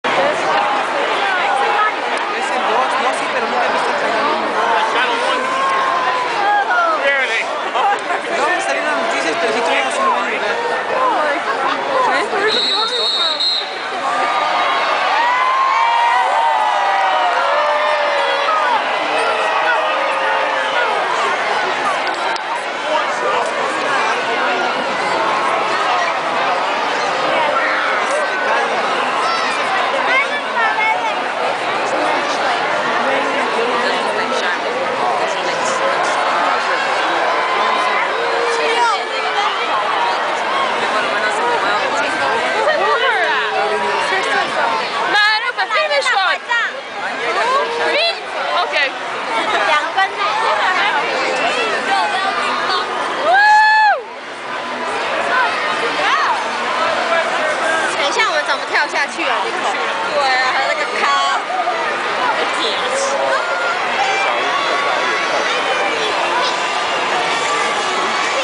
Kristin Chenoweth at the Macy's Thanksgiving Day Parade
Pushing Daises and Broadway Wicked star Kristin Chenoweth at the 2008 Macy's Thanksgiving Day Parade.